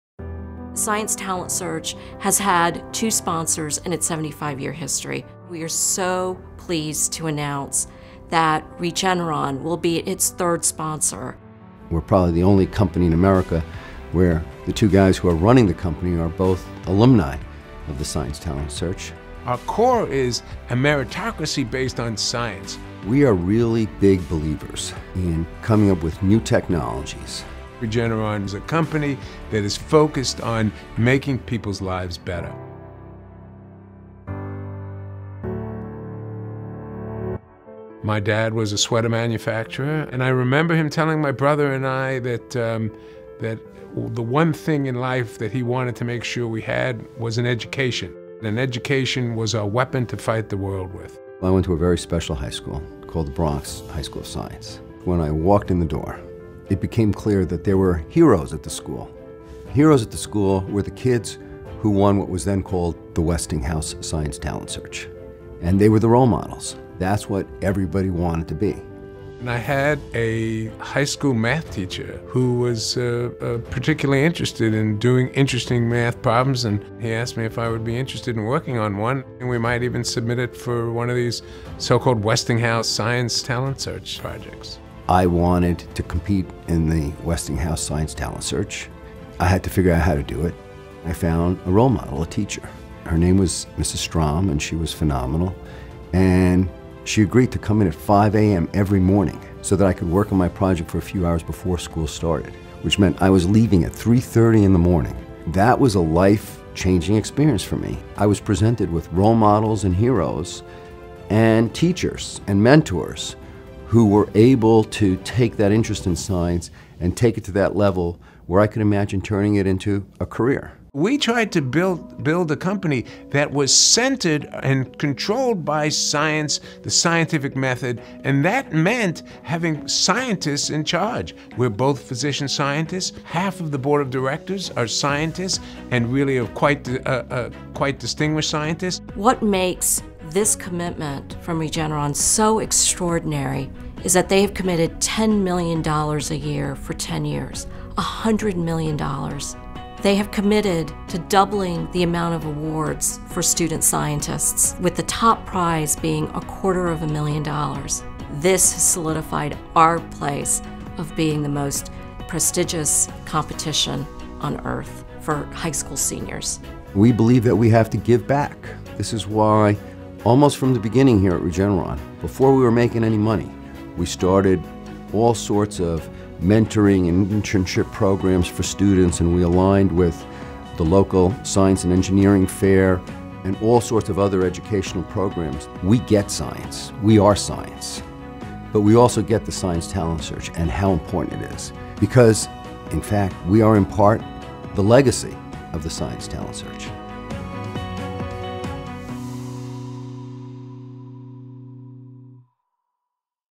STS Sponsorship Announcement
We’re honored and so excited to announce the Regeneron Science Talent Search. With their ten year commitment of $100 million, Regeneron becomes only the third sponsor in the 75 year history of the Science Talent Search. to learn more.